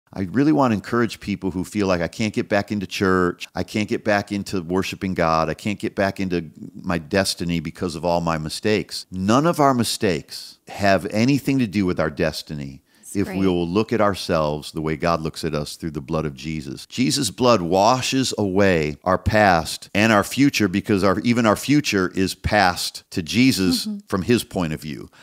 Your past has no power...
This is the beauty of intimacy with God. God doesn't relate to us on the basis of our behavior. He relates to us on the basis of Jesus' sacrifice. Your sins are washed away. He doesn't hold them against you.